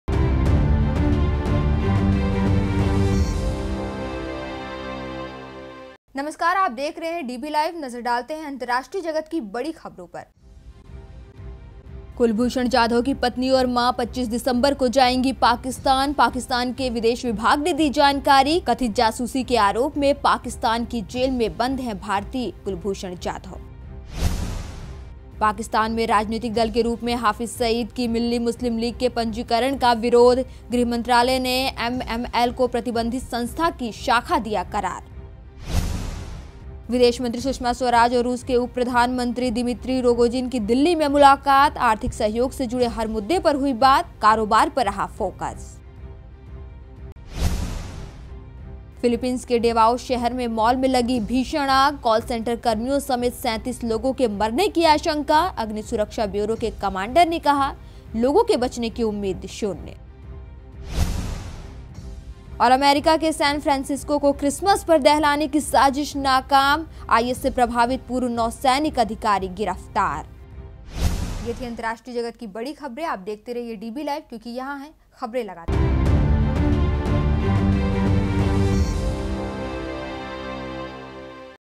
24 DECEMBER 2017 | अंतरराष्ट्रीय जगत की बड़ी खबरें | LATEST_INTERNATIONAL_NEWS
कुलभूषण जाधव की पत्नी और मां 25 दिसंबर को जाएंगी पाकिस्तान... पाकिस्तान के विदेश विभाग ने दी जानकारी... कथित जासूसी के आरोप में पाकिस्तान की जेल में बंद भारतीय कुलभूषण जाधव
पाकिस्तान में राजनीतिक दल के रूप में हाफिज सईद की मिल्ली मुस्लिम लीग के पंजीकरण का विरोध... गृह मंत्रालय ने एमएमएल को प्रतिबंधित संस्था की शाखा दिया करार
विदेश मंत्री सुषमा स्वराज और रूस के उप प्रधान मंत्री दिमित्री रोगोजिन की दिल्ली में मुलाकात... आर्थिक सहयोग से जुड़े हर मुद्दे पर हुई बात... कारोबार पर रहा फोकस
फिलीपीन्स के डेवाओ शहर के मॉल में लगी भीषण आग... कॉल सेंटर कर्मियों समेत 37 लोगों के मरने की आशंका... अग्नि सुरक्षा ब्यूरो के कमांडर ने कहा-लोगों के बचने की उम्मीद शून्य
अमेरिका के सैन फ्रांसिस्को को क्रिसमस पर दहलाने की साजिश नाकाम. IS से प्रभावित पूर्व नौसैनिक गिरफ्तार